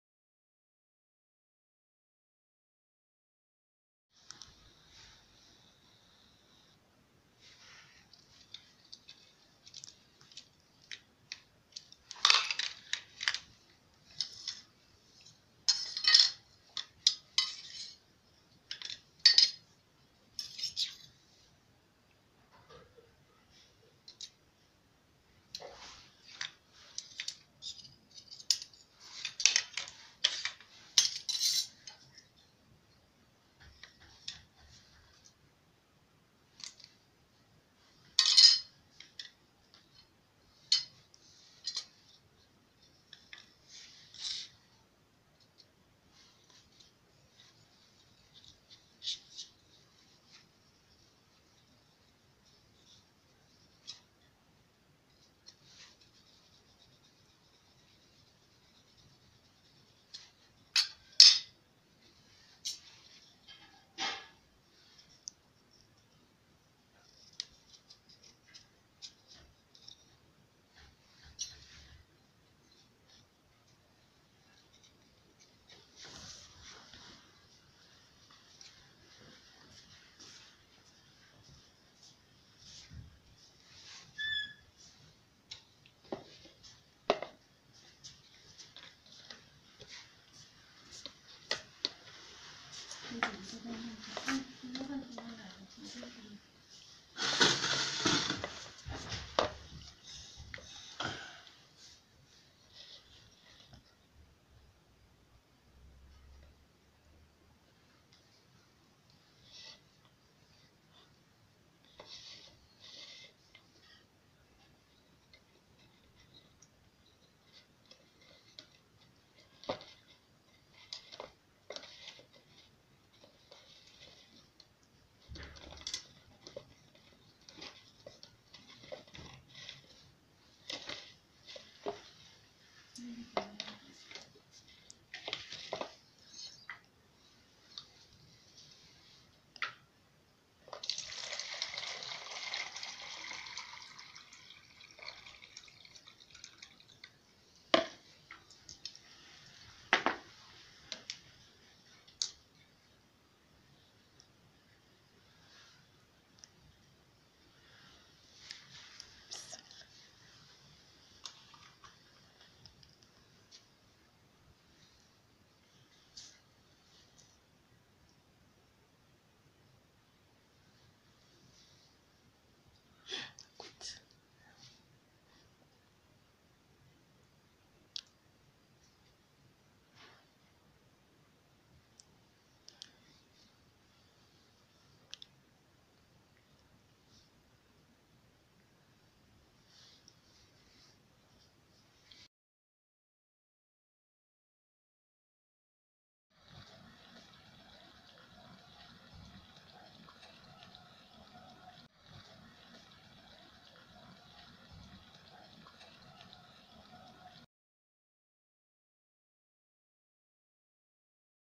How to Produce Electric Heater with 2 Spoons
This video is going to show you on how to produce Electric Heater with 2poons,
less time consuming,less power consuming, Less Electric Consuming, boil water within few minutes,